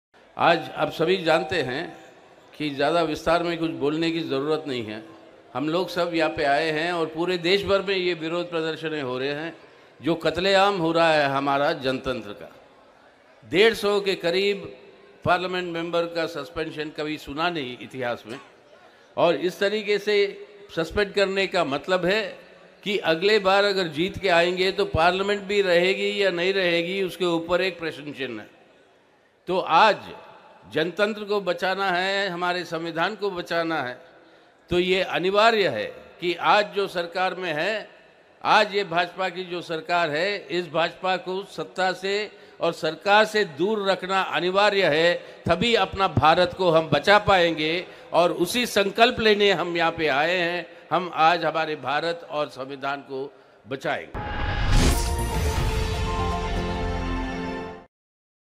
"never heard of such kind of suspensions" | INDIA Alliance | Protest | Sitaram Yechury
INDIA leaders protest against suspension of 143 INDIA parties MPs at Jantar Mantar, New Delhi